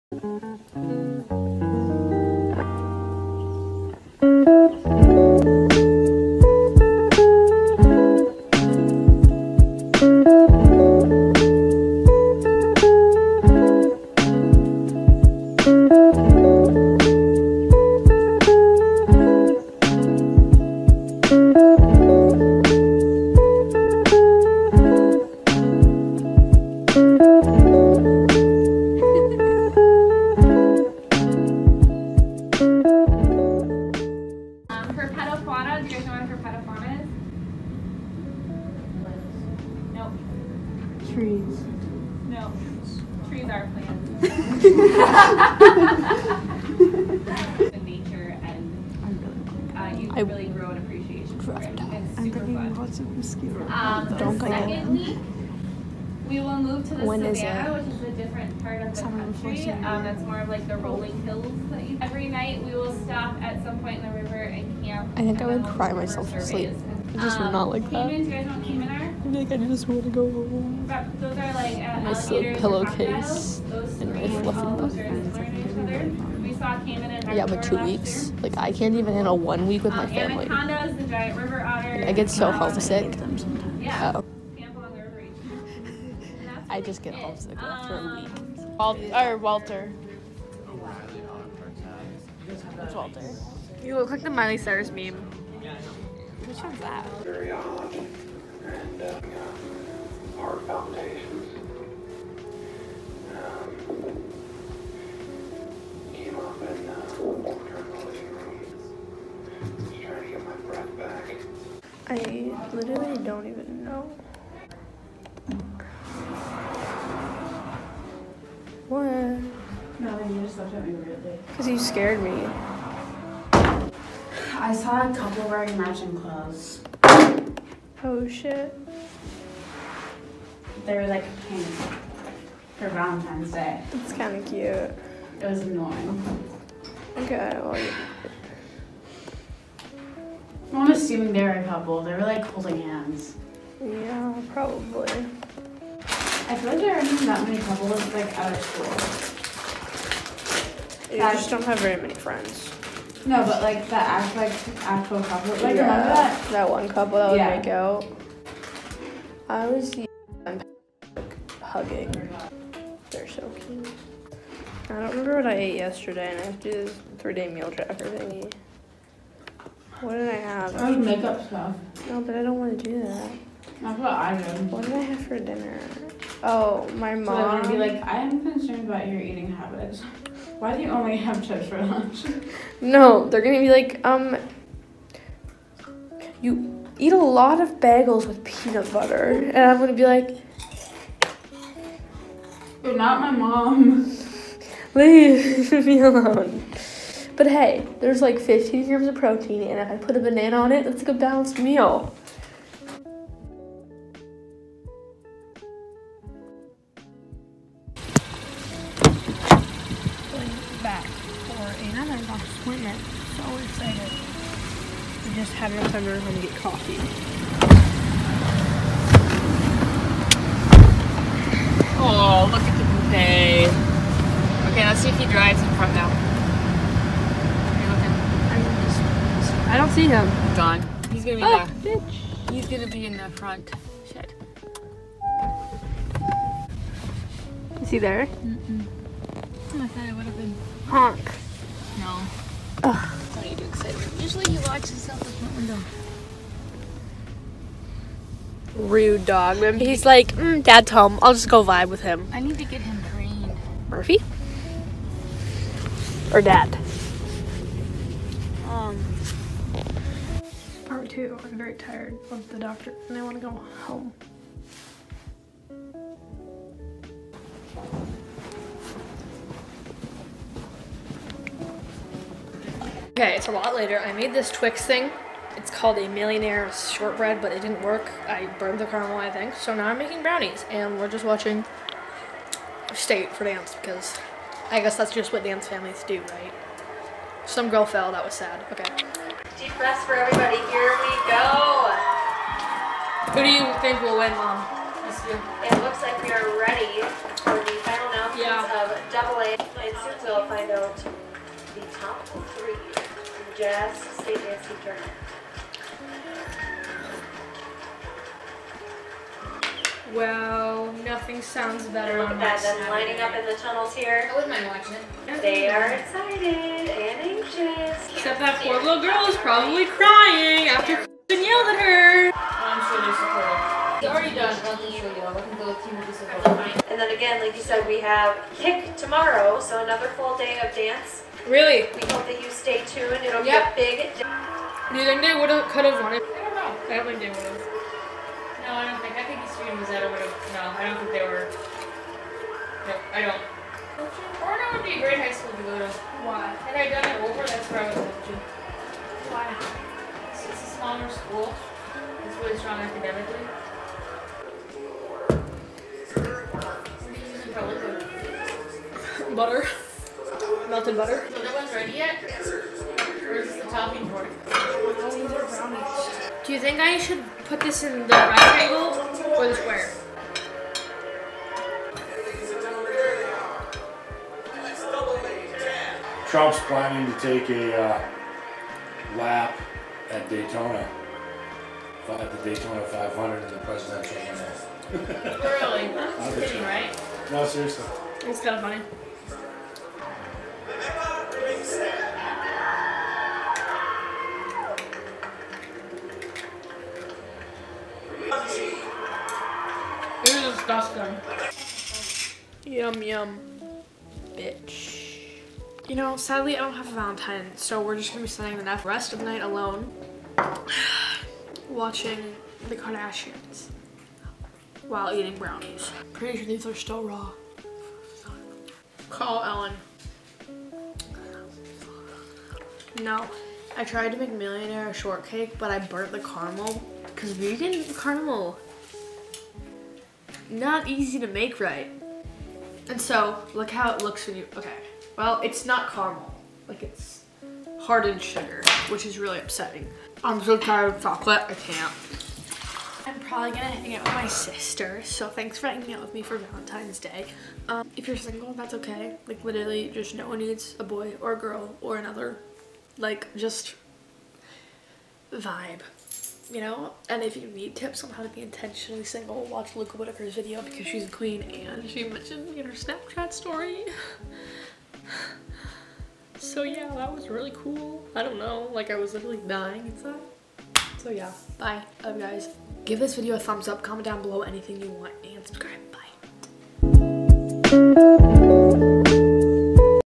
a lonely valentines day at school TOO MUCH CHOCOLATE HELP
Hi guys! In todays video we have a vlog of a day in my life in highschool on valentines day. I had two doctors appointments today and baked alone. I made millionaire shortbread and brownies (both vegan) while watching the state dance competition on tv (my sister did dance for many years on the team). I ate way to much chocolate!!!

ABOUT ME
age ~ 15
grade ~ sophomore
camera ~ nikon dx vr 18-55
editing software ~ sony vegas 16
locationa ~ minnesota hehe